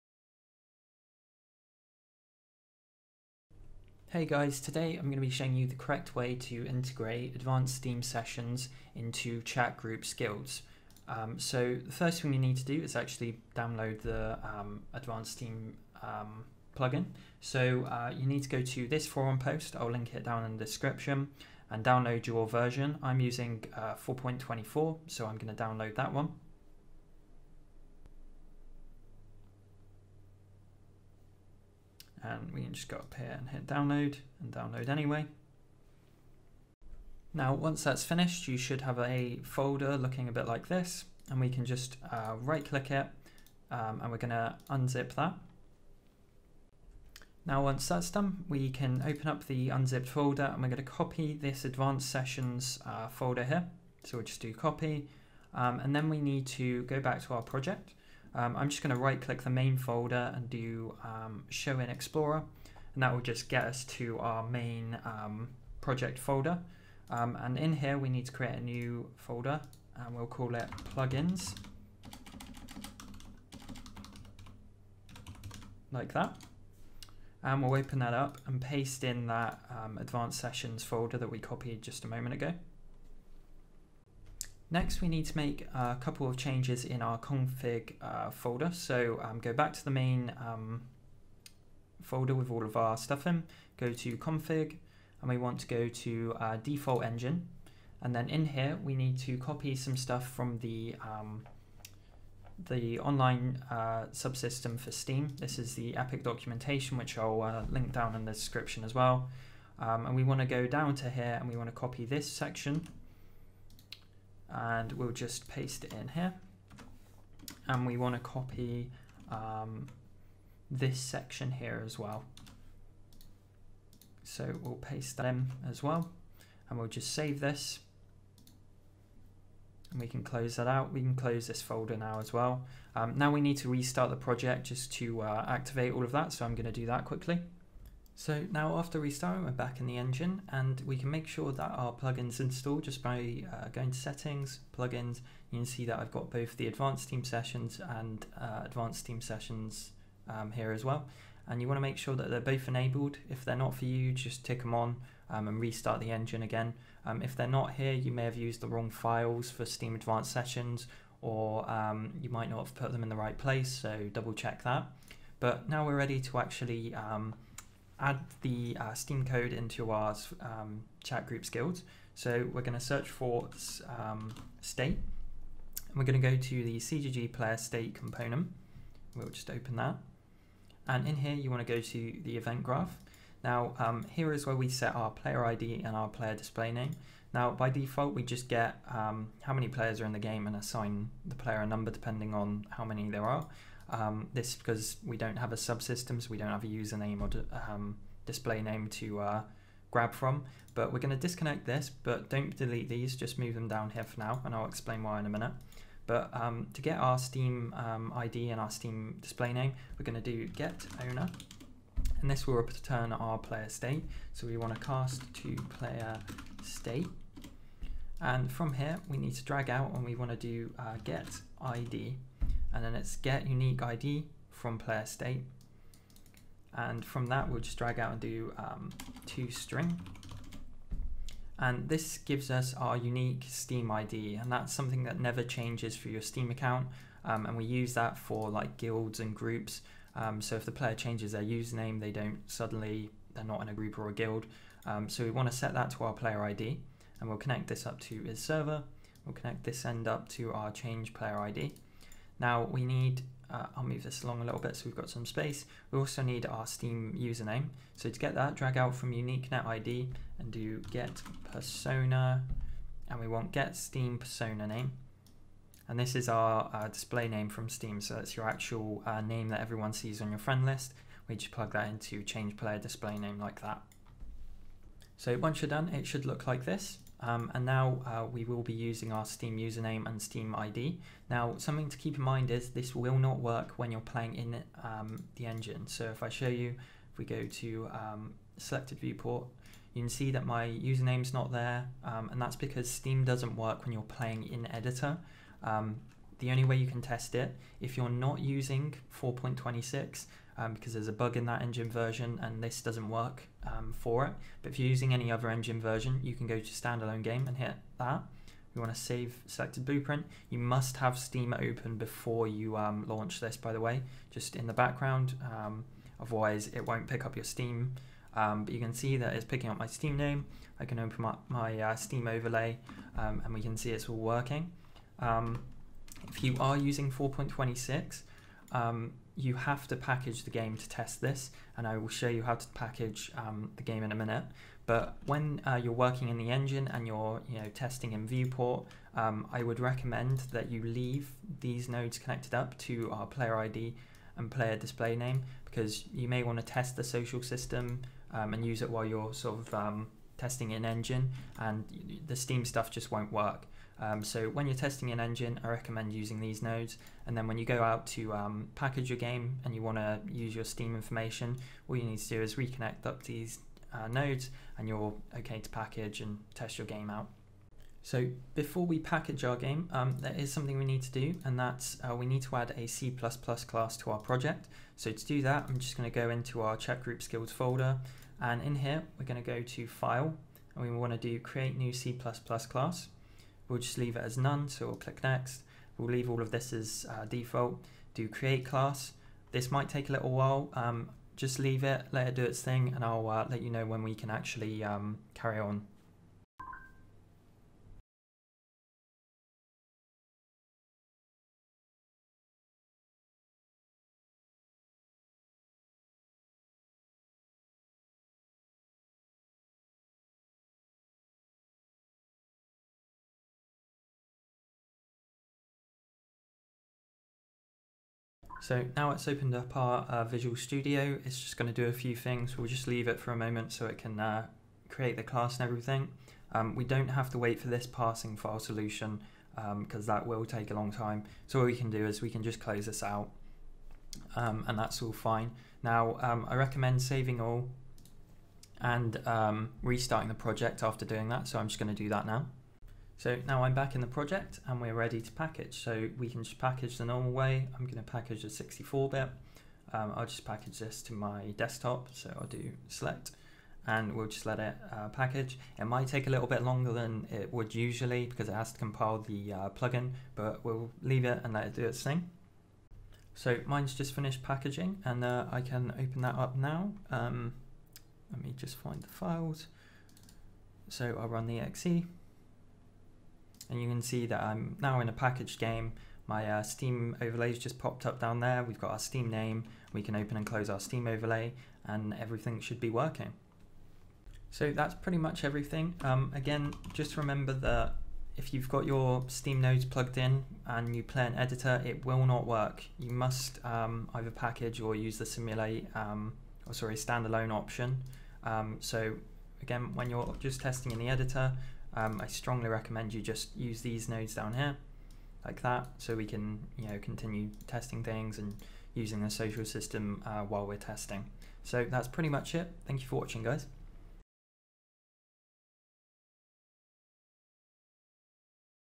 Chat Groups Guilds (Steam Integration)
This is a guide on how to use steam with CGG

Click▼

00:00 Installing Advanced Steam Sessions
02:17 Chat Groups Guilds Integration
06:07 Packaging Project
08:02 Using In UE4 Editor